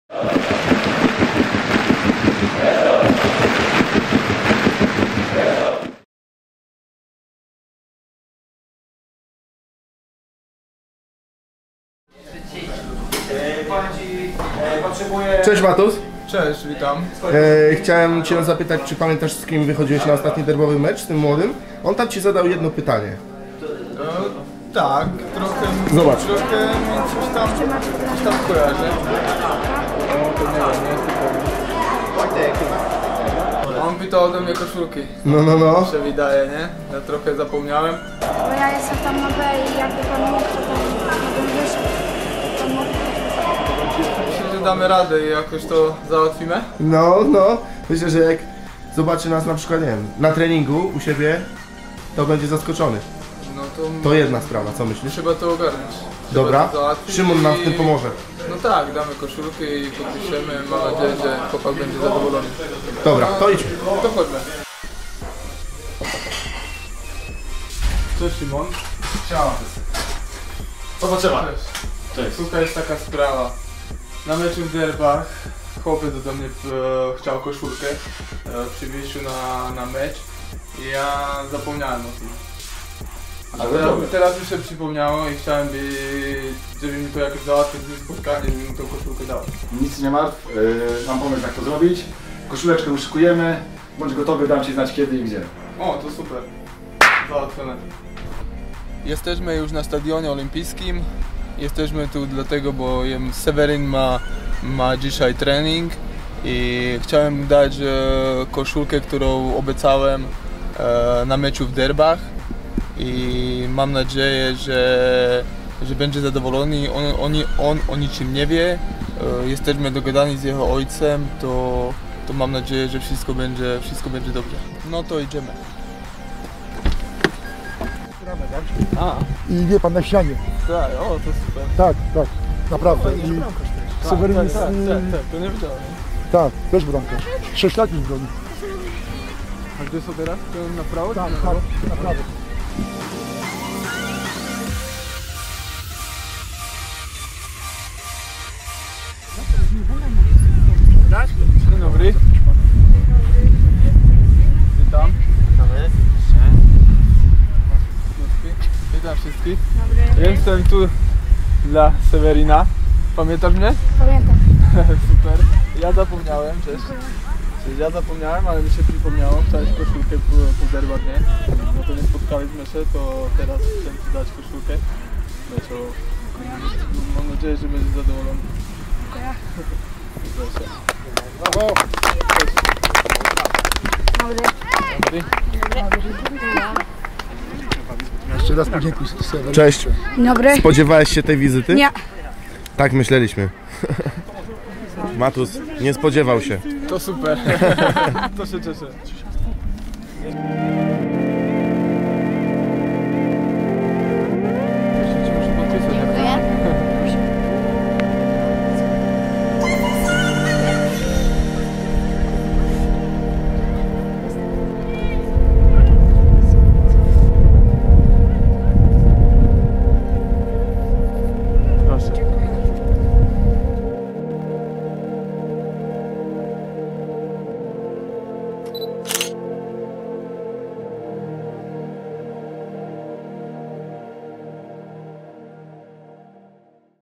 Matus, pamiętasz?
Jesteś młodym bramkarzem, wyprowadzasz na mecz swojego idola i pytasz go o koszulkę. Umawiacie się na "po meczu", a on... nie przychodzi. Co się dzieje dalej?

Idol Cię odszukuje, odwiedza na treningu i wręcza koszulkę z autografem! Zobaczcie fantastyczną historię Matusa i Seweryna.